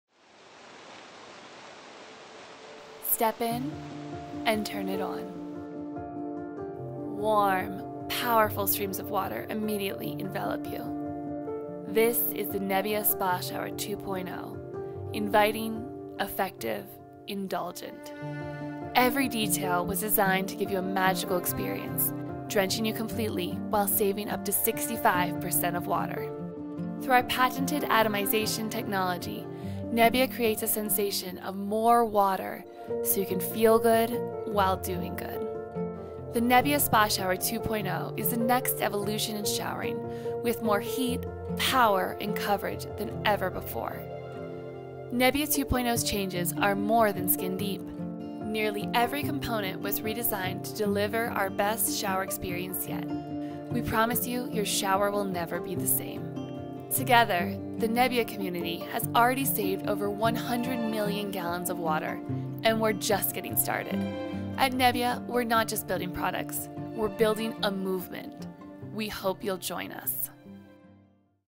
Step in and turn it on: Nebia 2.0
Introducing the Nebia Spa Shower 2.0. Now with more heat, power, and coverage than ever before.